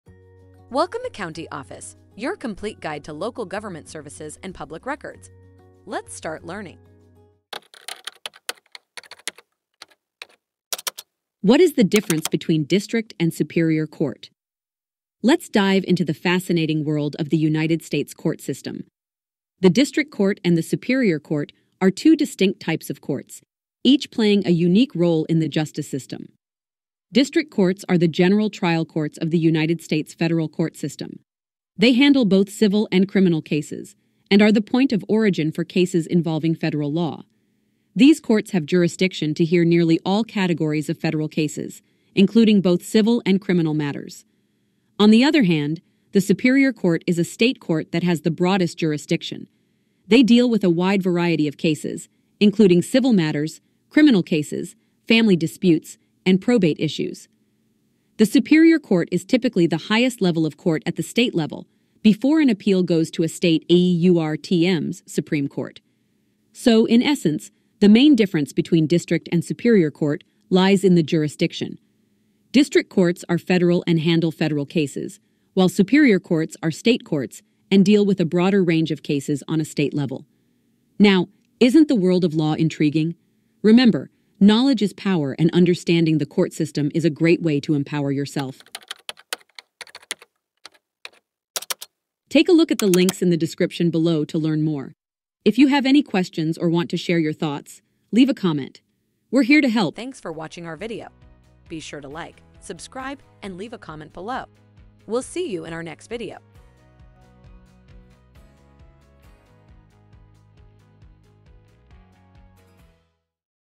What Is The Difference Between District And Superior Court - CountyOffice.org
What Is The Difference Between District And Superior Court Ever wondered how the US judicial system is structured and what differentiates one court from another? Dive into our latest video where we unravel the complexities of district courts and superior courts in the United States. Discover the unique roles these courts play in our legal system, from handling federal and state cases to the appointment of judges and the journey of appeals. Whether you're a law student, legal professional, or just curious about how justice is served, this video is your guide to understanding the intricacies of our courts.